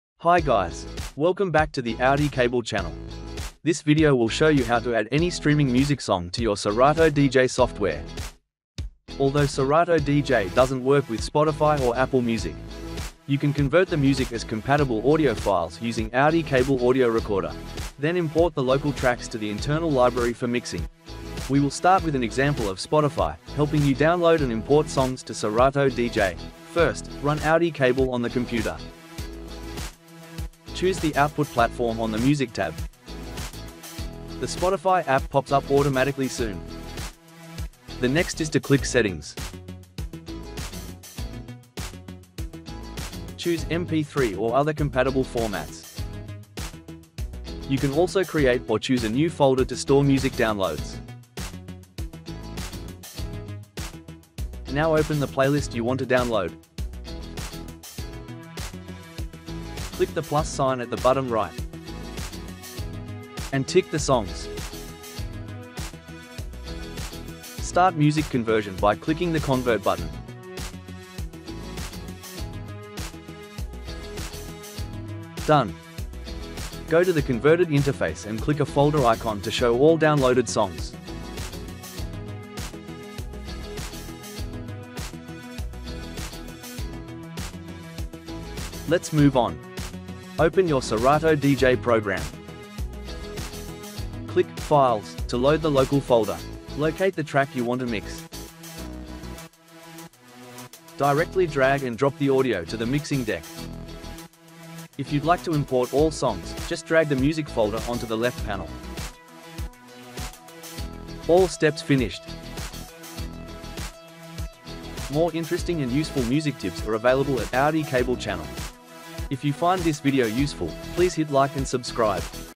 How to Add Any Music to Serato DJ - Work for Spotify/Apple Music/Amazon Music
Learn how to add any streaming music song to your Serato DJ Lite/Pro program in this video. AudiCable Audio Recorder is the key tool when you want to get Spotify, Apple Music, and Amazon Music tracks for mixing with Serato DJ.

AudiCable converts songs from various music platforms as local MP3/AAC/FLAC/WAV/AIFF/ALAC format, including Spotify, Amazon Music, Apple Music, YouTube Music, Tidal, etc.

Follow a few easy steps:
Step 1 Start AudiCable Audio Recorder and choose music platform.
Step 2 Choose the output format and output folder.
Step 3 Add songs from Amazon Music/Spotify/Apple Music Song to AudiCable.
Step 4 Check the converted songs in the local folder.
Step 5 Import the downloaded tracks to Serato DJ for mixing.